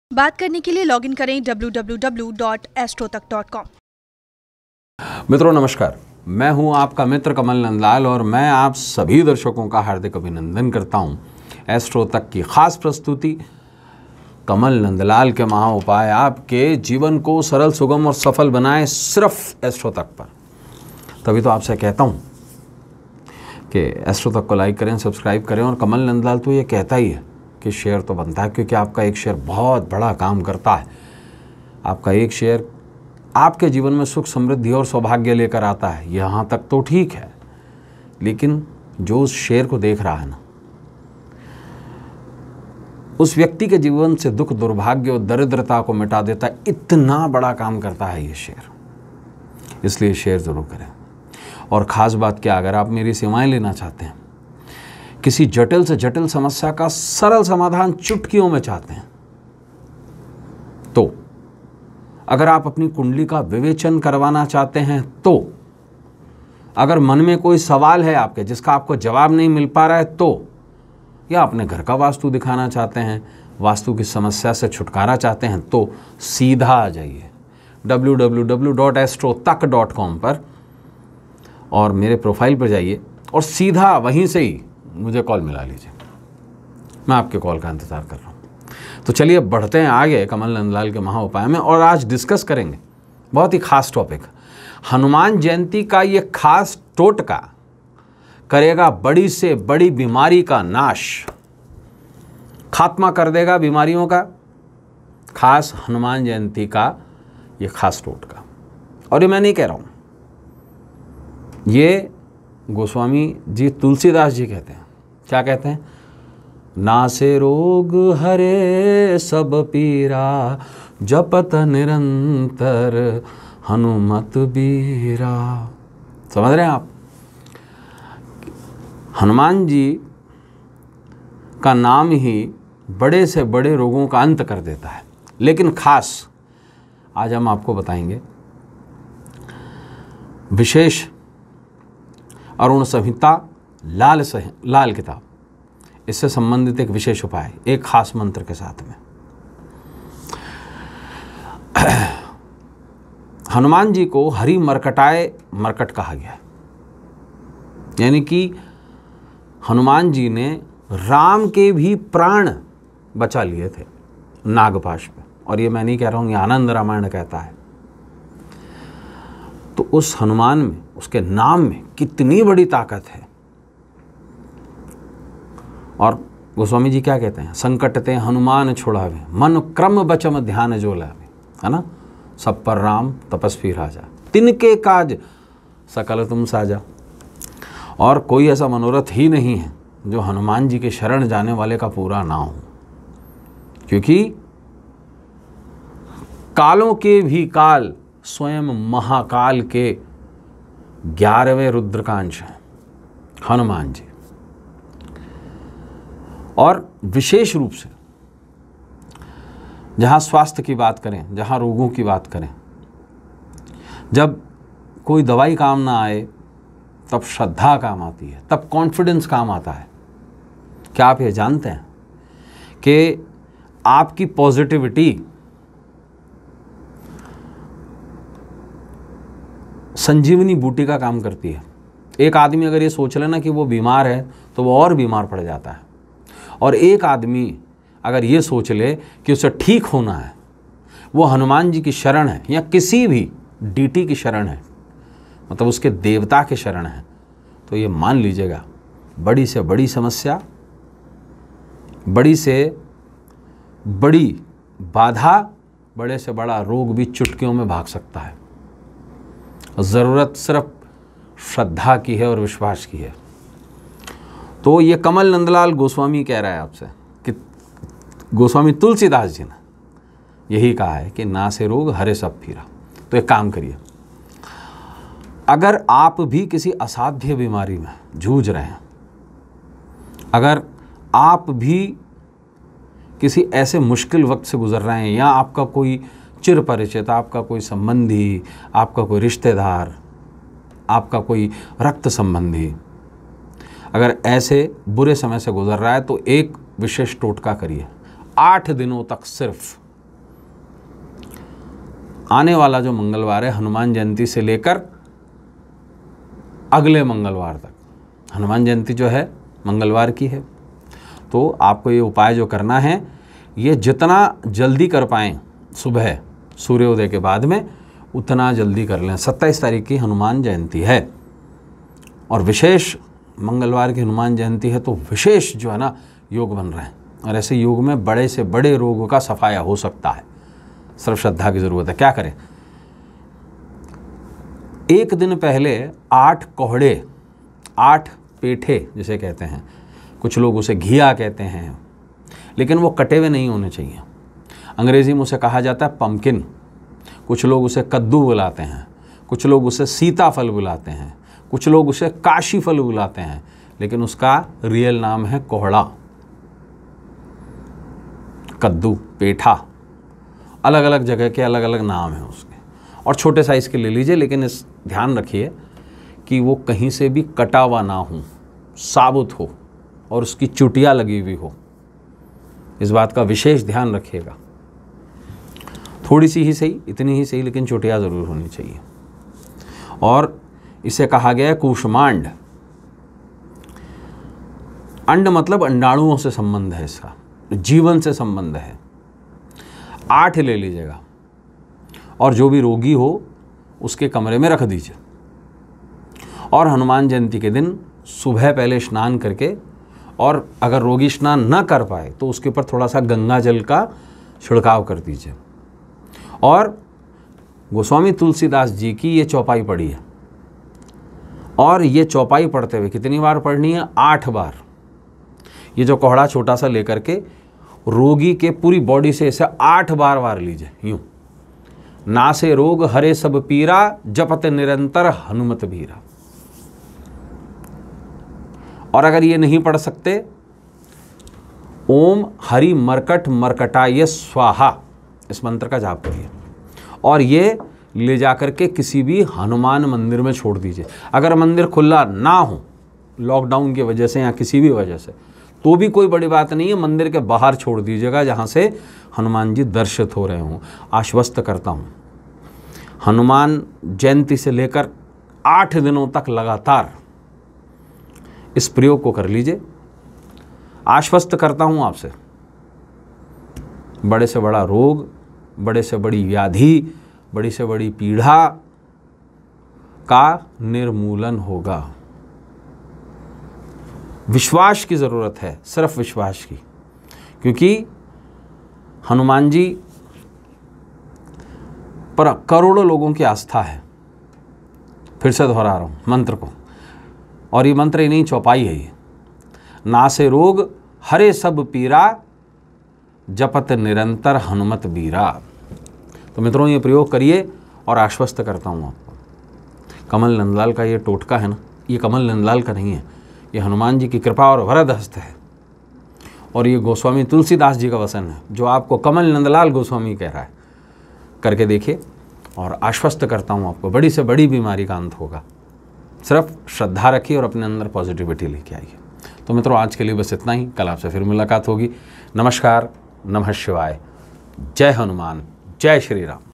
हनुमान जन्मोत्सव का ये खास टोटका करेगा सभी बीमारी का नाश | Kamal NandLal ke MahaUpay | Astro Tak
KAMAL NAND LAL के महाउपाय : ज्योतिर्विद कमल नंदलाल जी से हनुमान जन्मोत्सव के अवसर पर जानते हैं कि हनुमान जी का कौन सा खास टोटका करेगा बड़ी से बड़ी बीमारी का नाश...